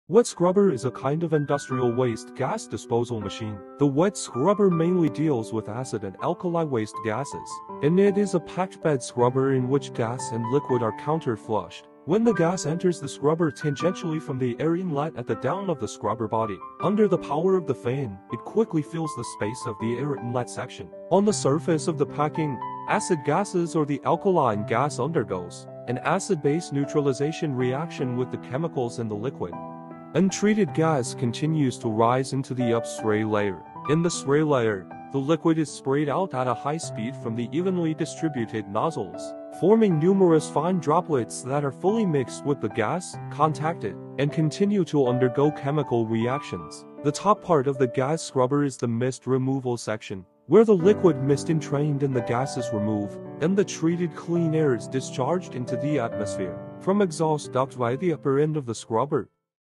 how wet scrubber work
A wet scrubber is a kind of industrial waste gas disposal machine.
The wet scrubber mainly deals with acid and alkali waste gases, and it is a packed bed scrubber in which gas and liquid are counter-flushed.
When the gas enters the scrubber tangentially from the air inlet at the down of the scrubber body, under the power of the fan, it quickly fills the space of the air inlet section, and then evenly rises into the filler layer.
On the surface of the packing, acid gases or alkaline gas undergo an acid-base neutralization reaction with the chemicals in the liquid.
The soluble substances generated by the reaction will flow into the water tank with the absorbing liquid.
Untreated gas continues to rise into the up spray layer.
In the spray layer, the liquid is sprayed out at a high speed from the evenly distributed nozzles, forming numerous fine droplets that are fully mixed with the gas, contacted, and continue to undergo chemical reactions.
Generally, our company Xicheng will use the form of a two-layer spray to complete this process to ensure that the exhaust gas can be fully reacted as much as possible.
The top part of the gas scrubber is the mist removal section, where the liquid mist entrained in the gas is removed, and the treated clean air is discharged into the atmosphere from the exhaust duct via the upper end of the scrubber.